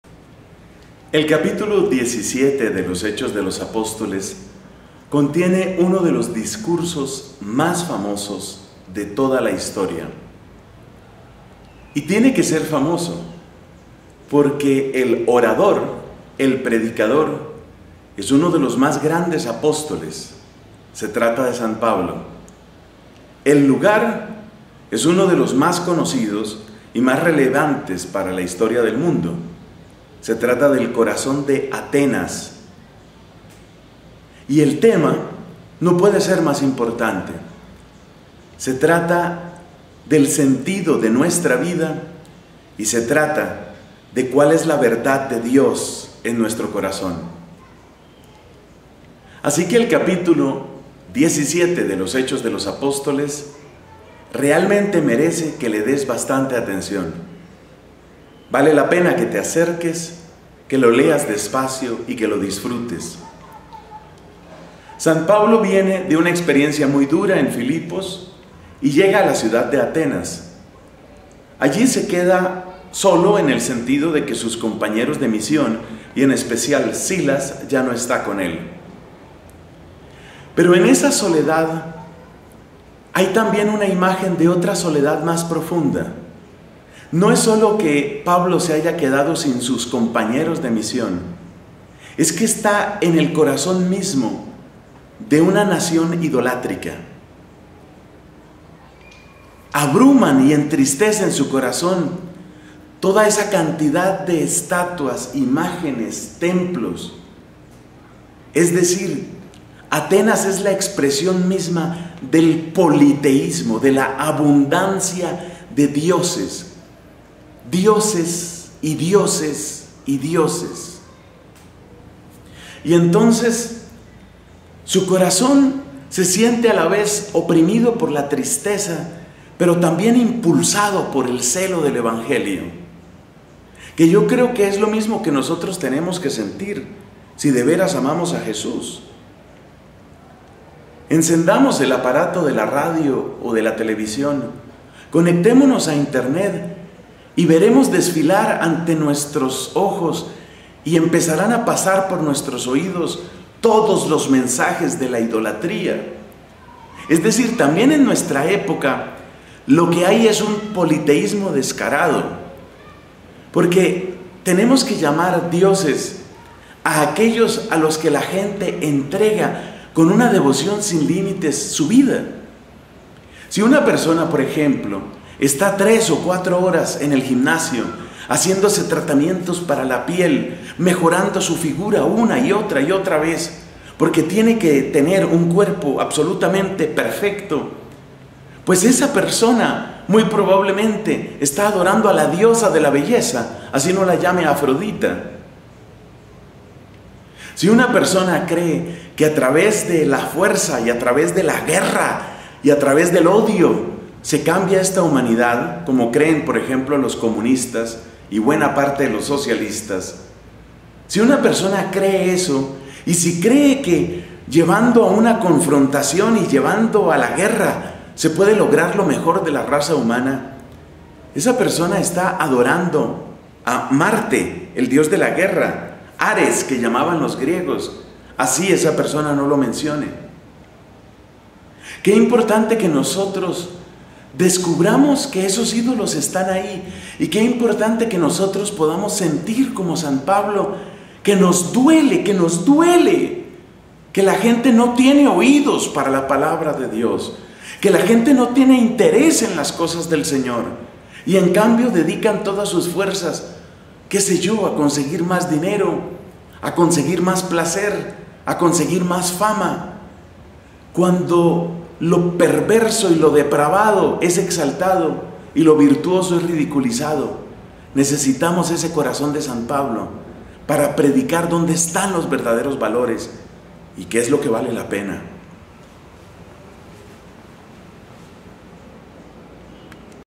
LA GRACIA del Miercoles 16 de Mayo de 2012
También hoy necesitamos del celo del apóstol Pablo porque también en nuestro mundo, como en la Atenas de Pablo, se multiplican los ídolos falsos.